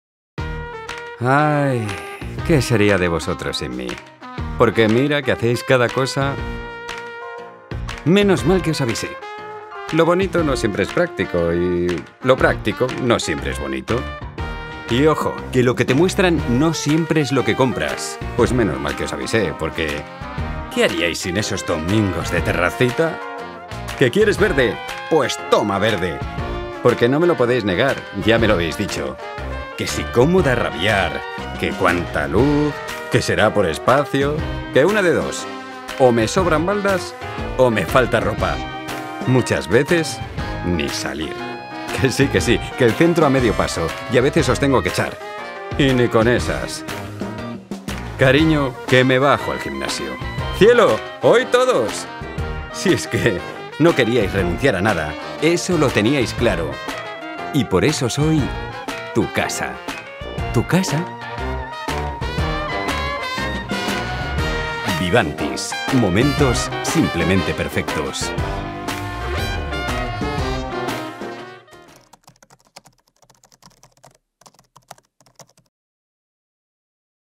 "Vivantis: Tu casa"
Viviendas Vivantis, viviendas pensadas para disfrutar de esos momentos simplemente perfectos.
Leer un libro o escuchar música en tu sillón favorito, mientras el sol acaricia tu cara. Tomarte esa cervecita en la terraza después del trabajo, mientras ves a tu hijo jugar en los jardines. Pasar un domingo en familia, viendo una peli con palomitas tumbados en el sofá. Celebrar comidas o cenas con tus amigos en casa, sin preocuparte de si faltará espacio. Ir al gimnasio a cualquier hora del día o jugar un partido de pádel con los amigos.
Cada vez que llego a mi casa, me siento feliz.